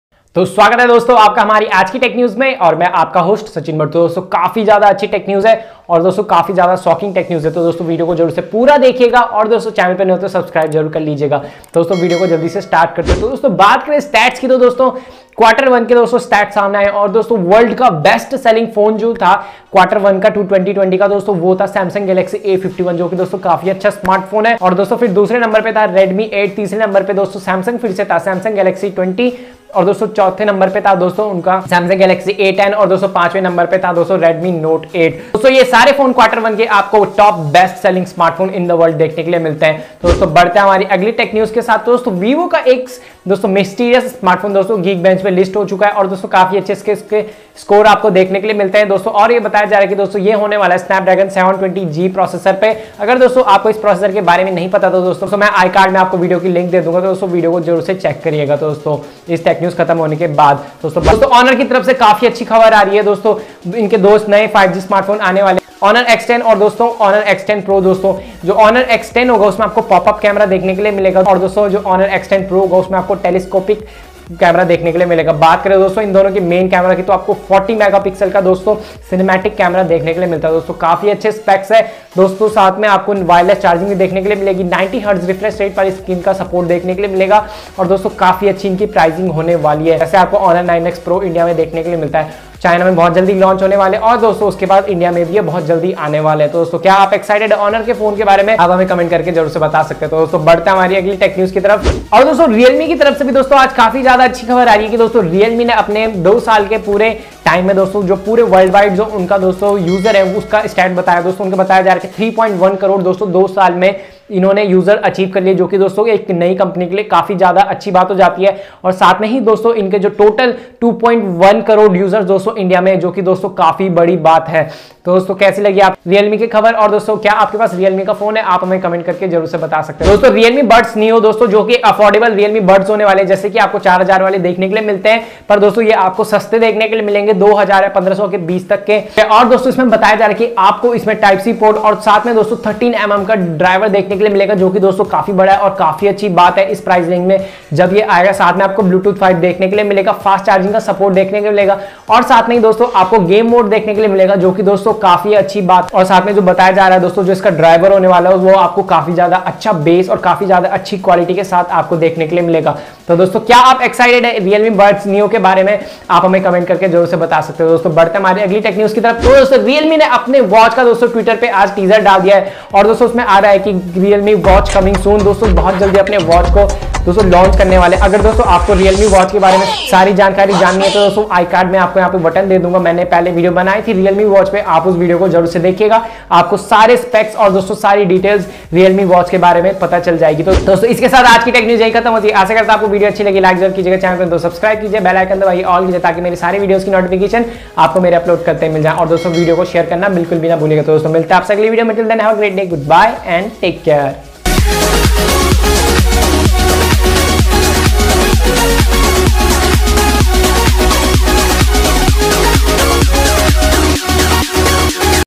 Realme Buds Neo 1500rs, realme watch official look, Honor X10 & X10 Pro 5G India launch, Vivo Z1 5G😲
Follow me on the Facebook by liking and following my Facebook page"Sachin Bhatt Knowledge's Power ".

This video is about = Today's Tech News

Sachin Bhatt